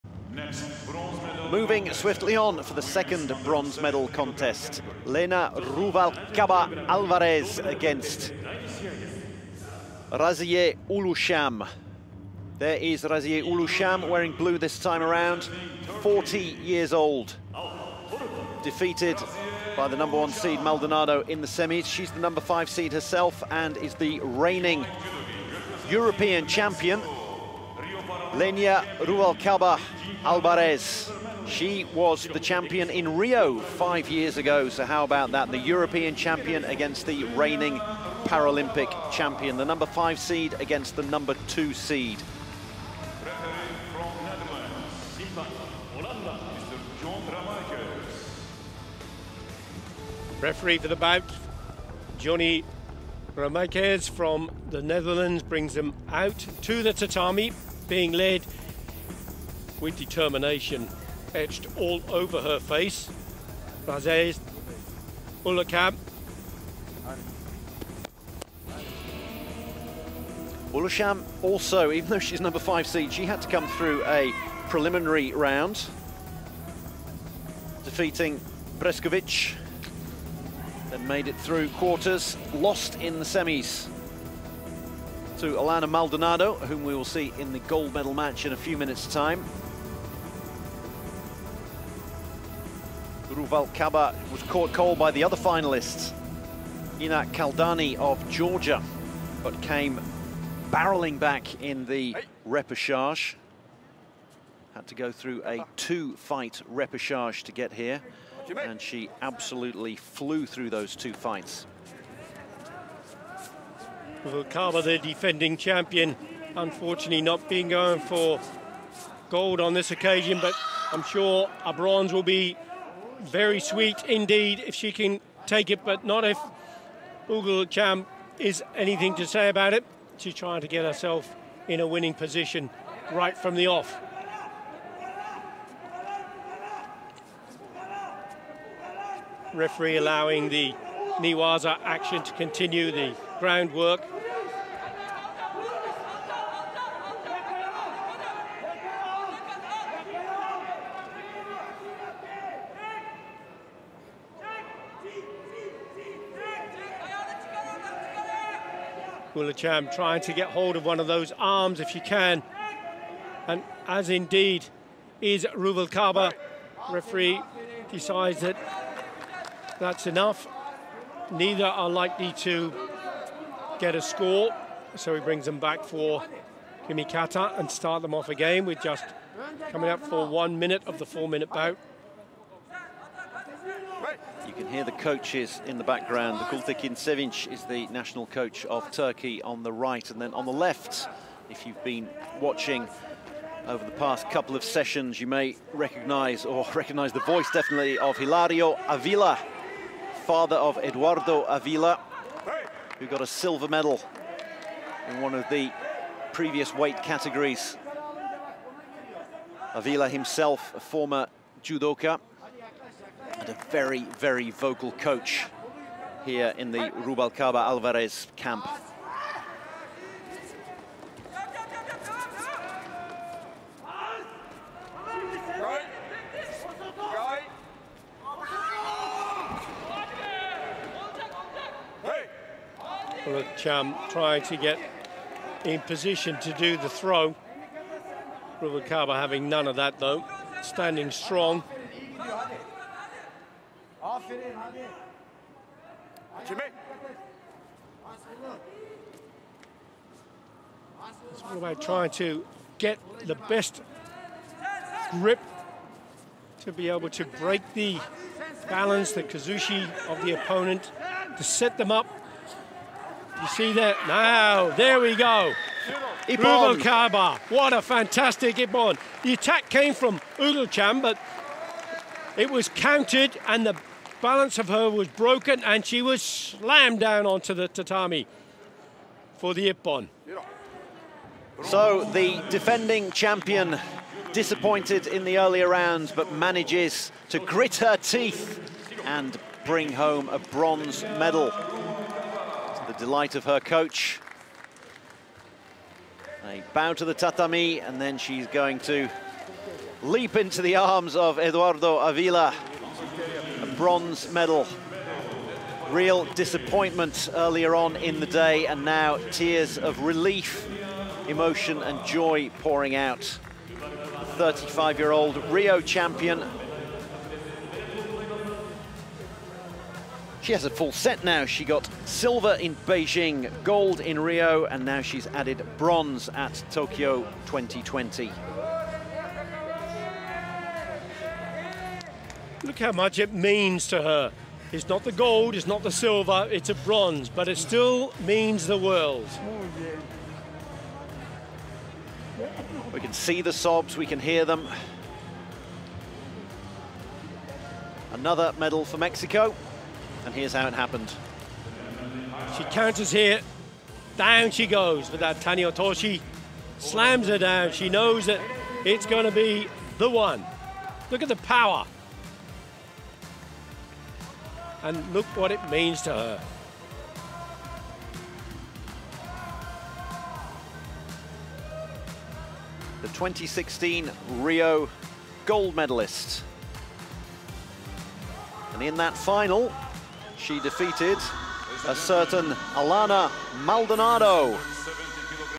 Bronze Medal for Lenia Ruvalcaba | Women's B3 +70kg Bronze Medal Match |Judo |Tokyo 2020 Paralympics
Mexico's Lenia Fabiola Ruvalcaba Alvarez wins the women's B3 +70kg bronze medal match.